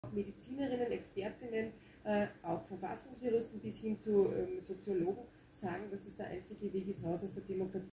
Raus aus der Demokratie
Quelle Oe24tv: Edtstadler: „Impfpflicht ist einzige Weg raus aus der Demokratie“